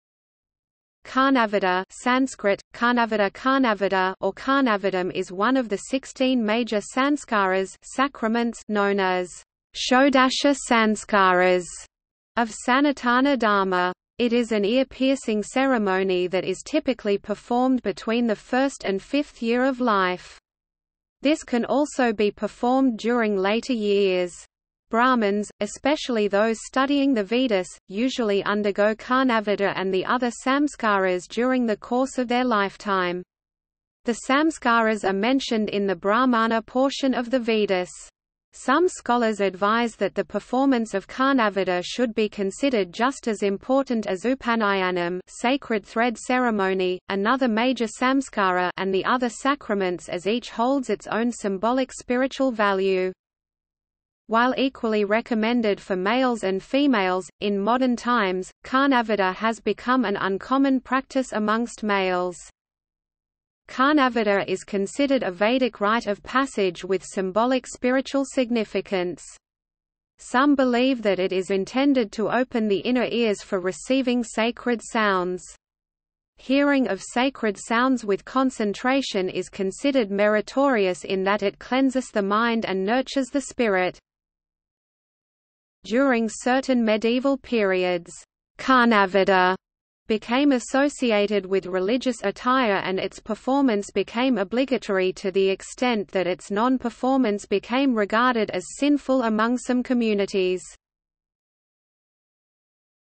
Karnavedha | Wikipedia audio article
- Socrates

SUMMARY
Karnavedha (Sanskrit: कर्णवेध, Karṇavedha) or  Karnavedham is one of the sixteen major sanskaras (sacraments) known as "Shodasha Sanskaras" of Sanatana Dharma.  It is an ear piercing ceremony that is typically performed between the first and fifth year of life. This can also be performed during later years. Brahmins, especially those studying the vedas, usually undergo karnavedha and the other samskaras during the course of their lifetime. The samskaras are mentioned in the  Brahmana portion of the  vedas. Some scholars advise that the performance of karnavedha should be considered just as important as upanayanam (sacred thread ceremony - another major samskara ) and the other sacraments as each holds its own symbolic spiritual value.
While equally recommended for males and females, in modern times, karnavedha has become an uncommon practice amongst males.
Karnavedha is considered a vedic rite of passage with symbolic spiritual significance. Some believe that it is intended to open the inner ears for receiving sacred sounds. Hearing of sacred sounds with concentration is considered meritorious in that it cleanses the mind and nurtures the spirit.
During certain medieval periods, "karnavedha" became associated with religious attire and its performance became obligatory to the extent that its non-performance became regarded as sinful among some communities.